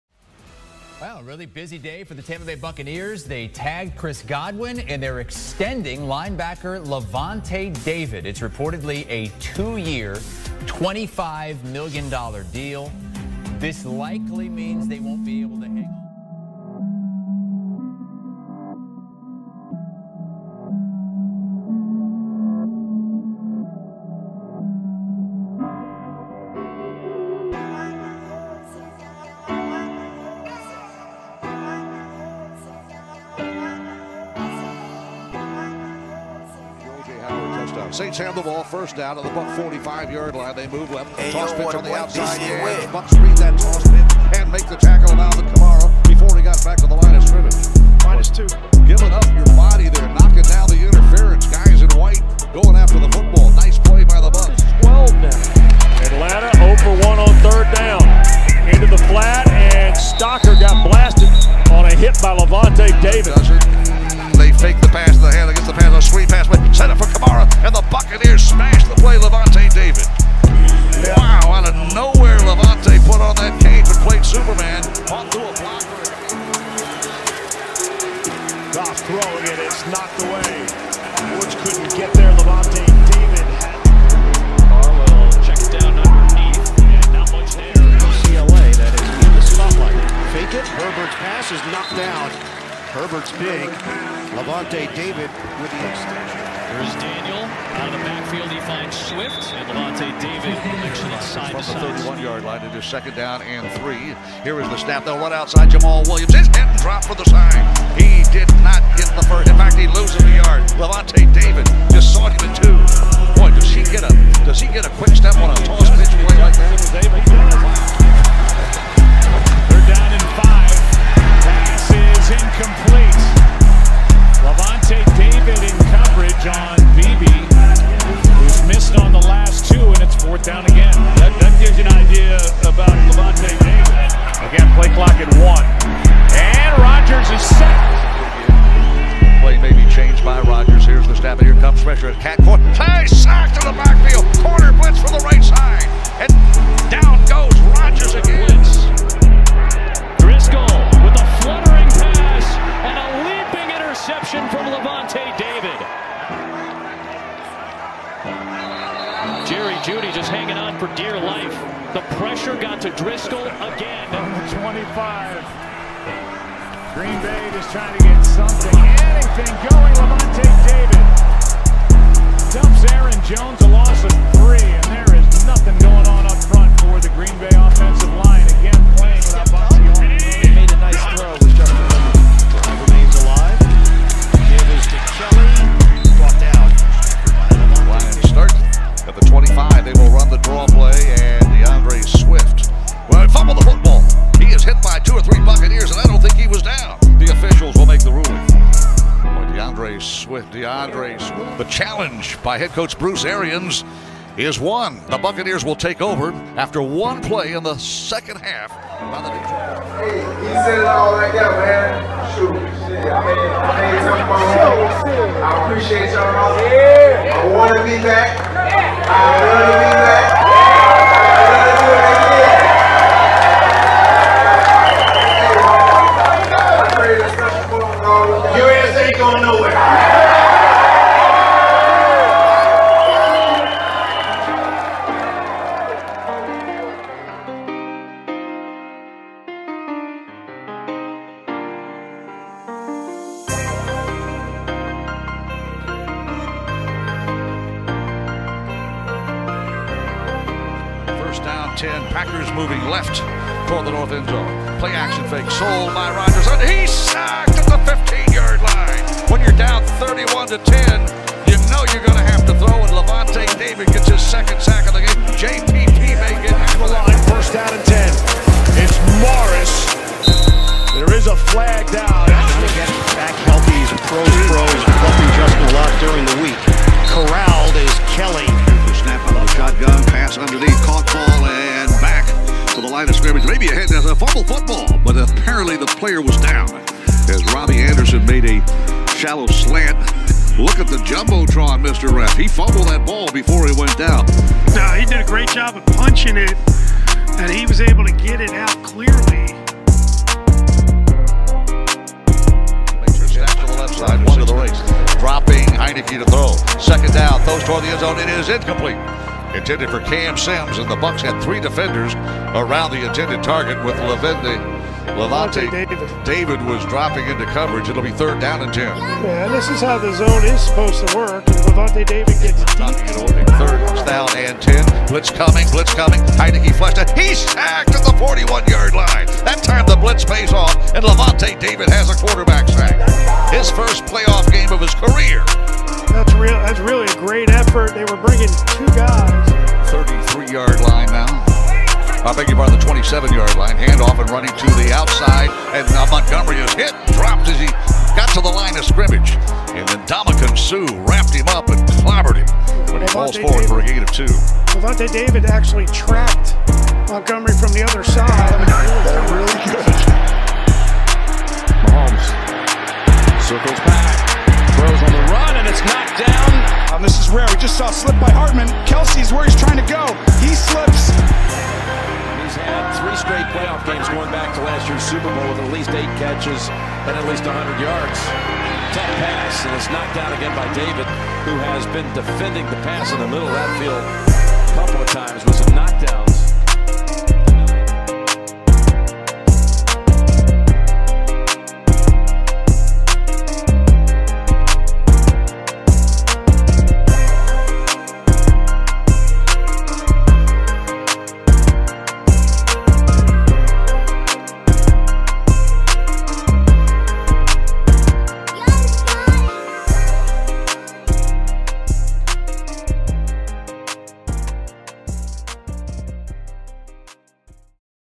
Lavonte David || 2020-2021 Season Highlight Mix || Tampa Bay Buccaneers
Highlight video for All-pro captain linebacker Lavonte David during his 9th NFL Season with the Super Bowl 55 champion Tampa Bay Buccaneers!